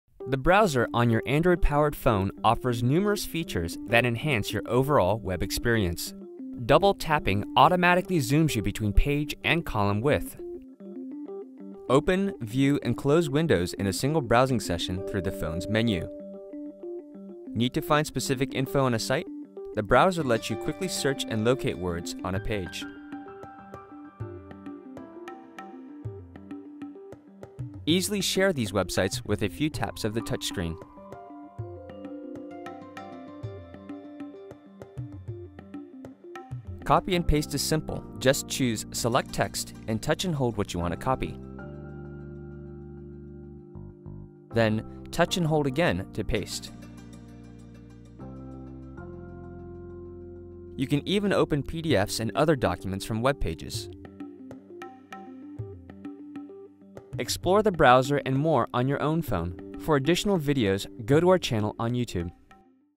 Browser (2.0)
Learn how the Android browser enhances your overall web experience.   Please note that this video is for devices running Android 2.0